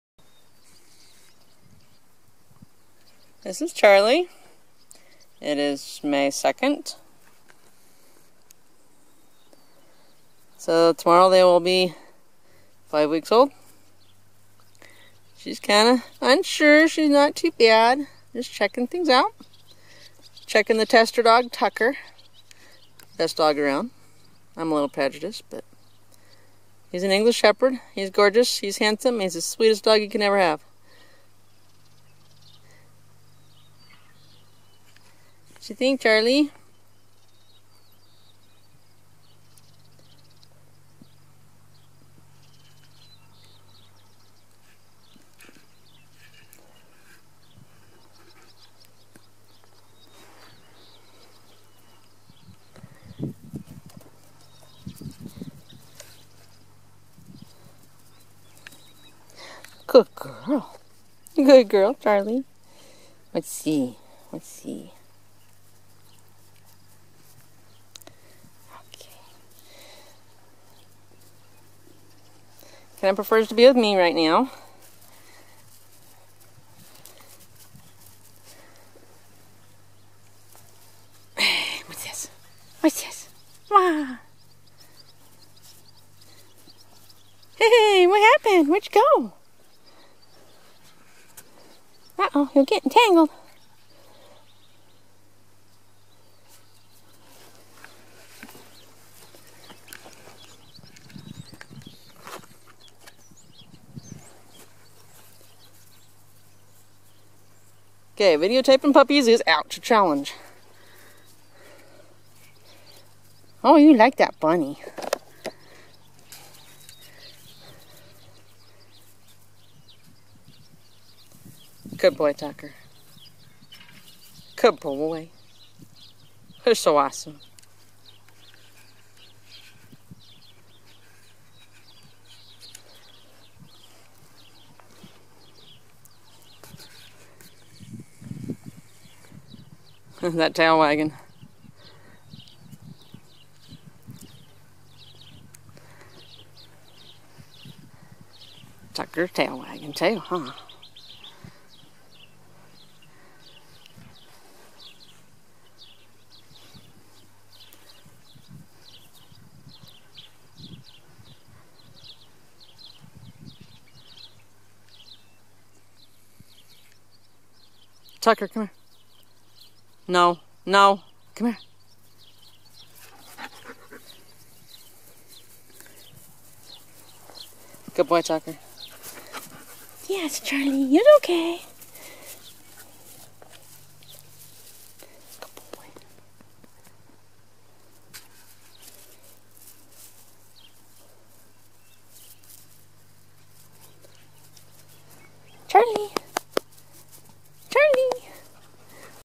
Charli - Rat Terrier pup -5 weeks old
Not sure why I said May 2, it was actually May 12. But anyway, this is little Charli. A little unsure at first, but quickly became more interested in exploring. Nice size pup, good temperament.